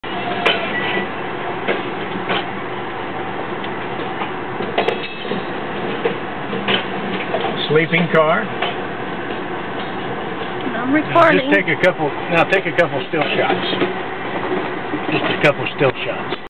TSA SHOULD BE CHECKIN' PASSENGER'S JUNK AUTOTRAIN Sanford Fla.
Up-Close and personal with Amtrack  --  another Government success---like Medicare   WAY TOO CLOSE for Video, unless you've had a TSA agent Handle your JUNK First.